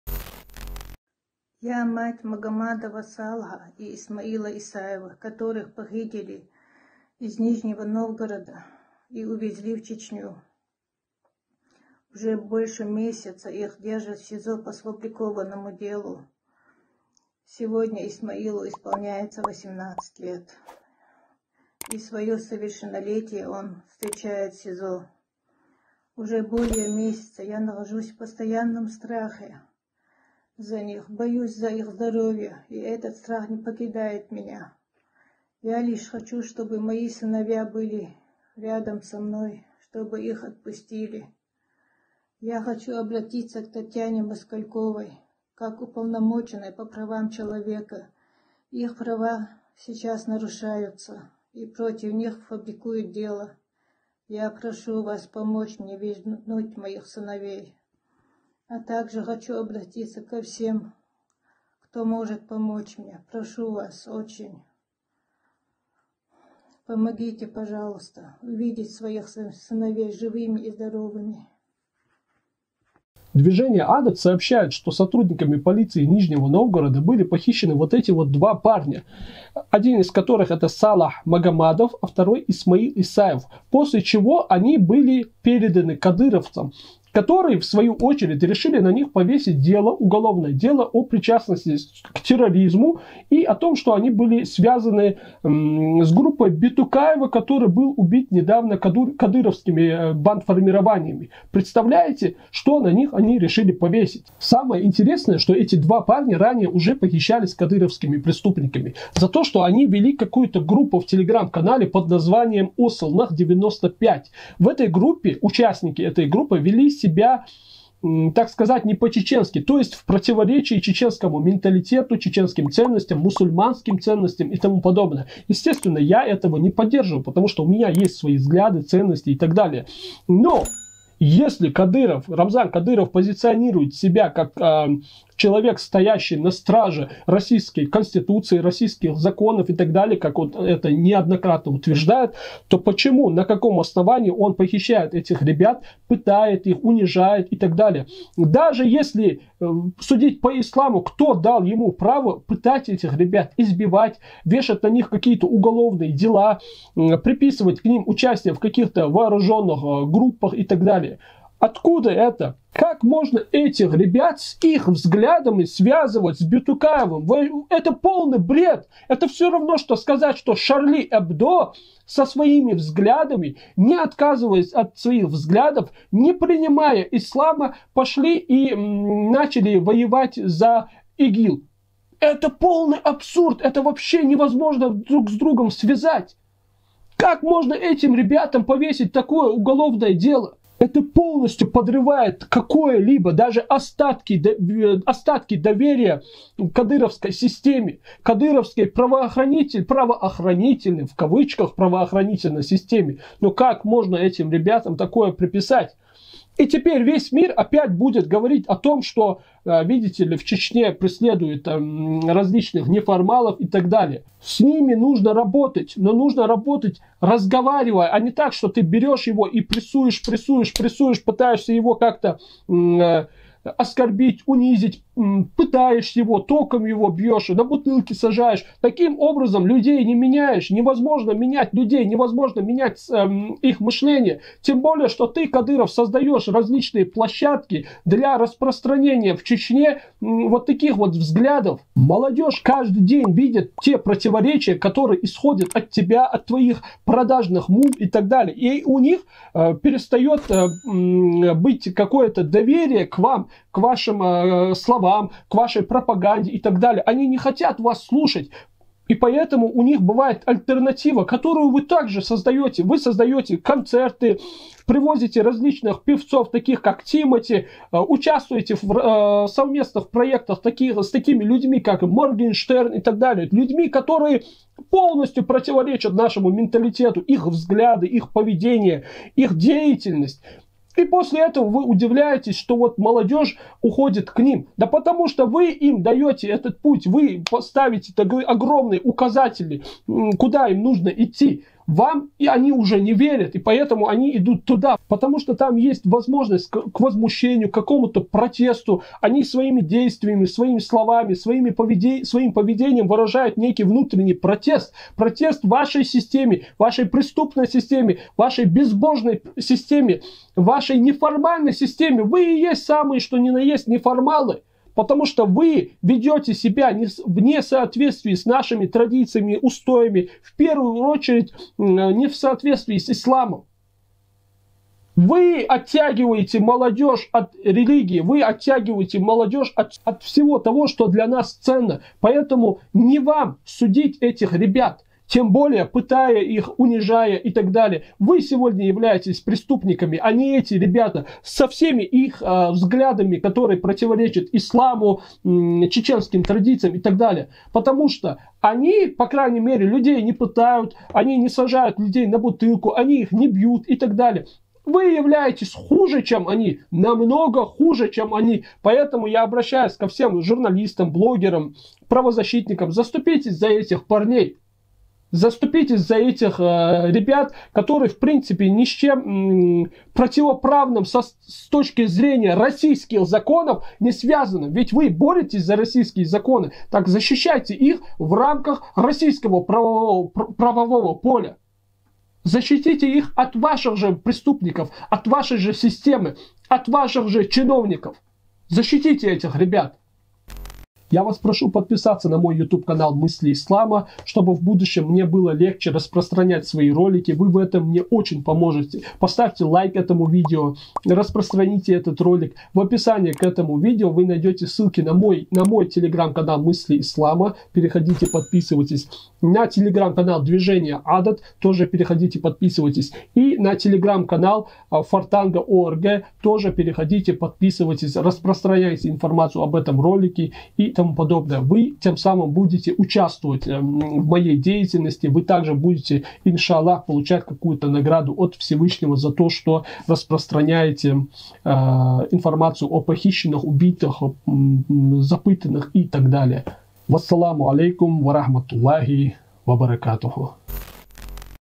ОБРАЩЕНИЕ МАТЕРИ ПОХИЩЕННЫХ САЛАХА И ИСМАИЛА
⭐️СТАТЬЯ НОВОЙ ГАЗЕТЫ - «Я служил в чеченской полиции и не хотел убивать людей»